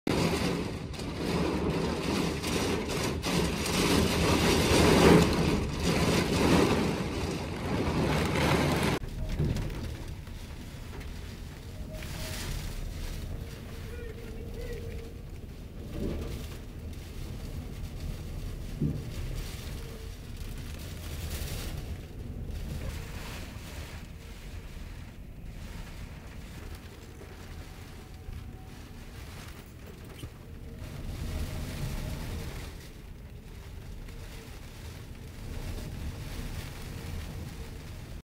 East coast crazy weather. Shot with Filmic Pro Double take
This video was shot with iPhone 11 and Double take App from filmic Pro
Man was fighting the heavy winds at Lowes  parking lot.